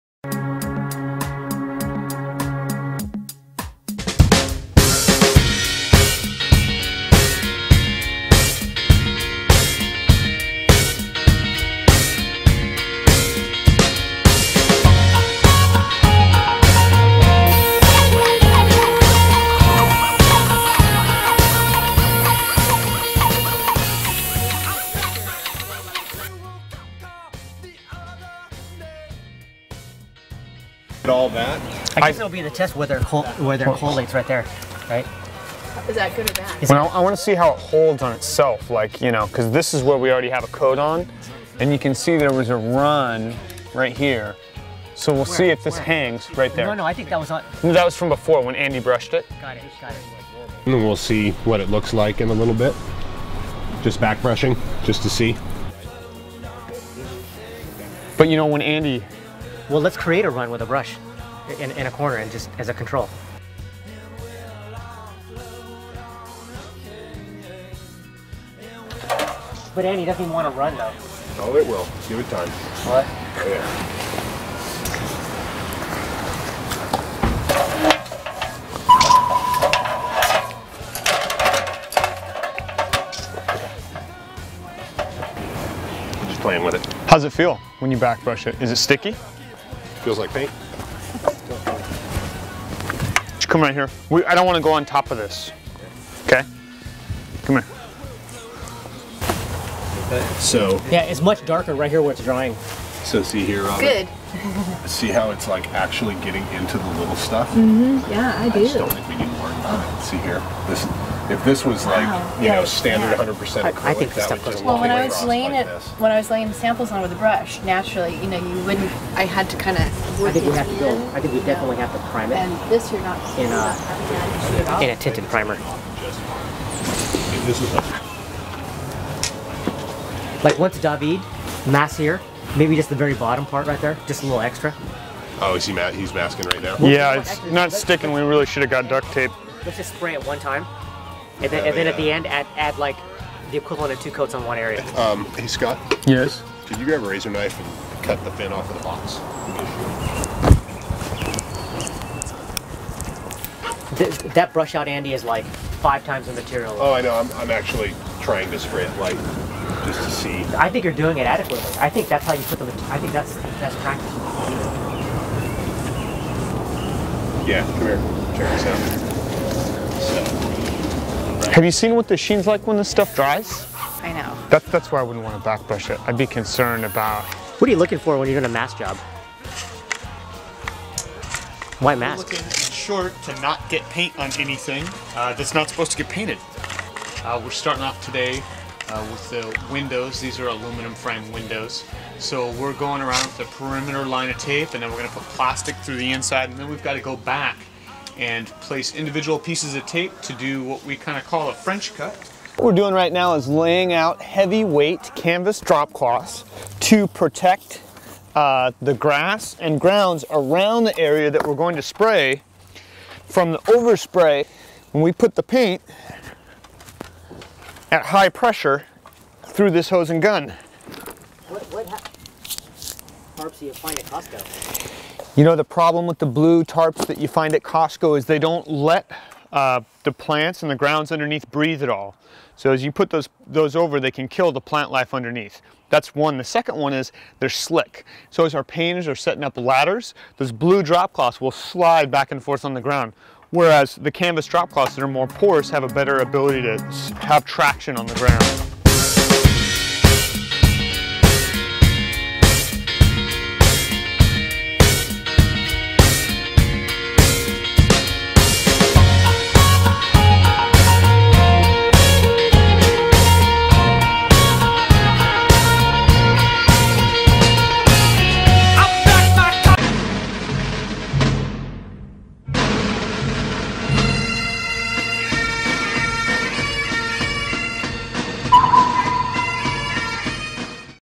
House Painting Cost with Fine Paints of Europe
This video demonstrates the application of Eco from Fine Paints of Europe on a single family residence in Seattle Washington by Shearer Painting. More information about Eco paint can be found
The cost of Eco is approximately 2.5 times the cost of Benjamin Moore Aura ex Benjamin Aura on this house would have cost $1800; Fine Paints of Europe $4500